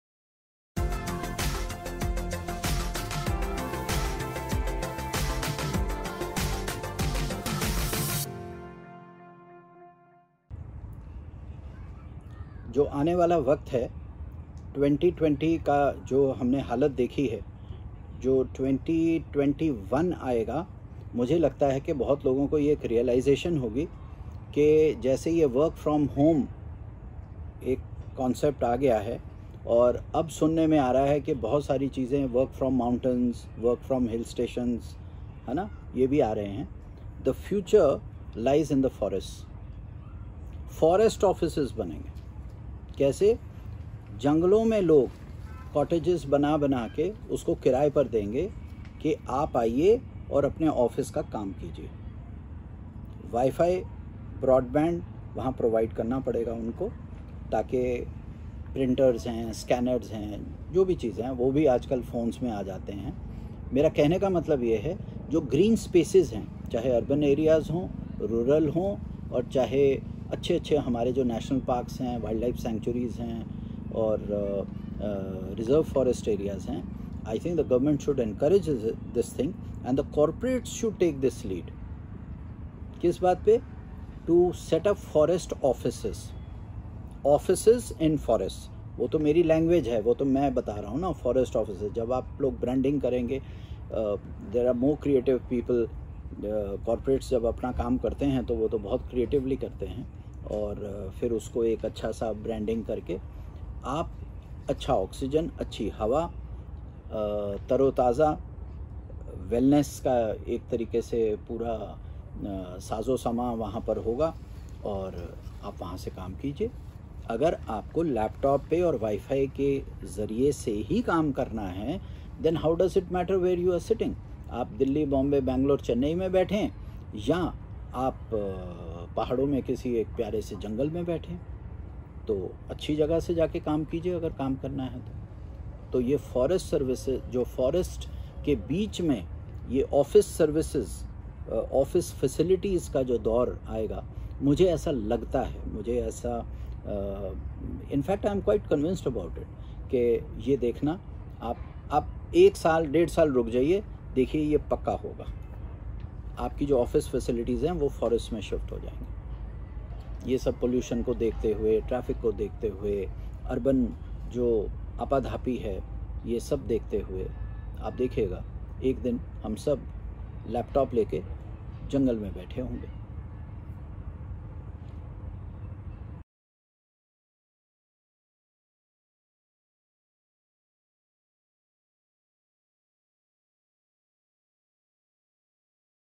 Is working from forests the future of corporate work? | Peepal Baba
Peepal Baba explains how forests can be the future of working from home.

Give Me Trees Trust has planted more than 20 million trees all across India. It was founded by Peepal Baba, who is one of the most renowned environmentalists in the country. Give Me Trees has more than 11,500 volunteers throughout the country.